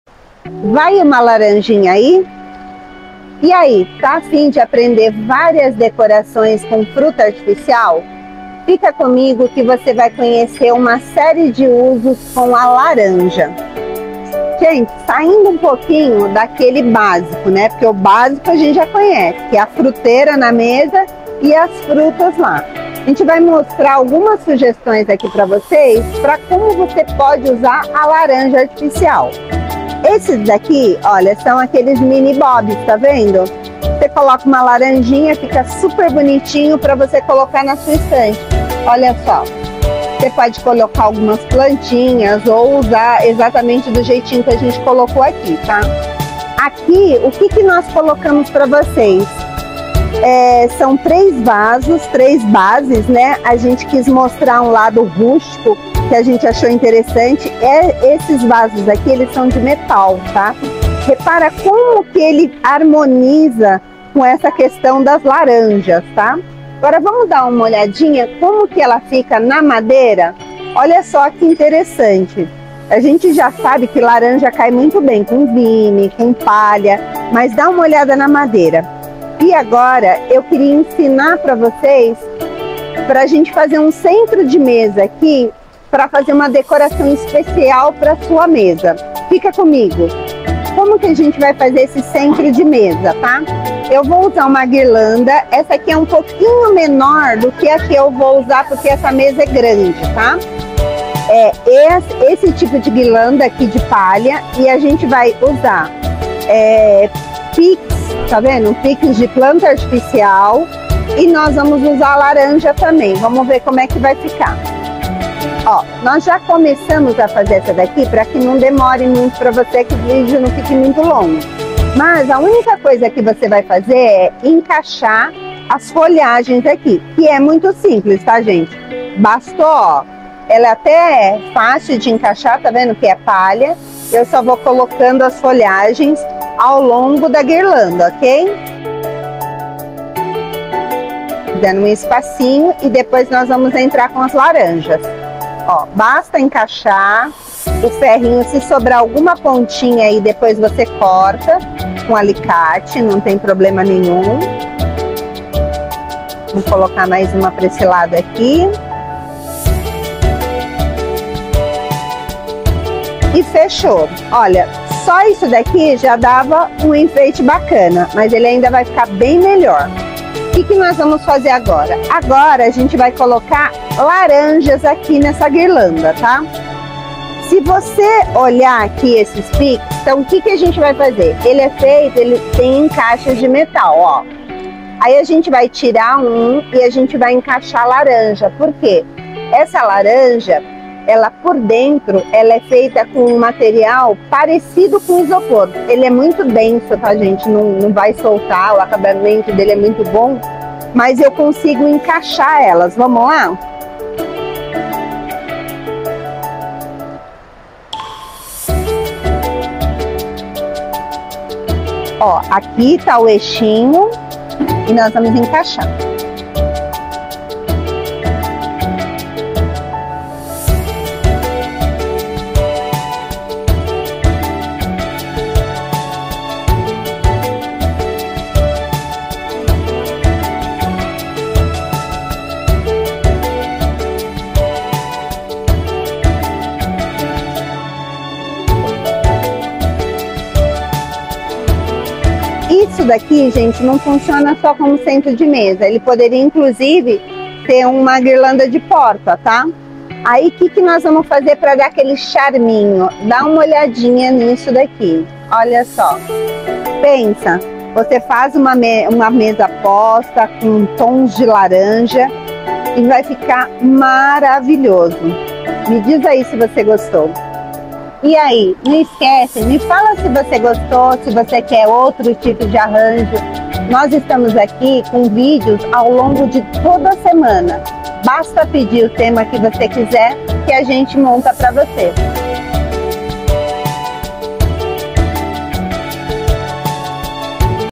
Aprenda A Decorar Com Laranja Artificial - Decoração com frutas permanentes
Gosta de decorações coloridas que fogem do tradicional? Então este vídeo é para você!

Nossa linha de frutas artificiais possui um modelo mais bonito que outro e no vídeo de hoje vamos te ensinar a decorar utilizando nossas laranjas artificiais, você pode estar pensando que é muito difícil decorar com frutas permanentes mais vai se surpreender com as diferentes maneiras de uso.

Faça você também e nos envie o resultado, deixe nos comentários o que você achou do nosso vídeo e até a próxima.

Já conhece a nossa loja ou já comprou nossos produtos, avalie a sua experiência no link abaixo, se quiser pode inserir fotos